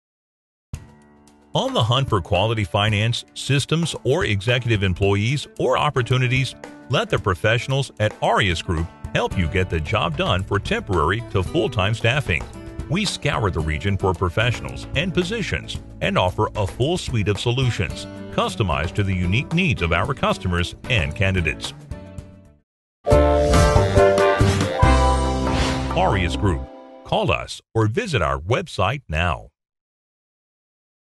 Aureus Group
On the hunt for quality finance, systems or executive
employees or opportunities? Let the professionals at Aureus
Group help you get the job done for temporary to full time
staffing. We scour the region for professionals and
positions and offer a full suite of solutions, customized to
the unique needs of our customers and candidates.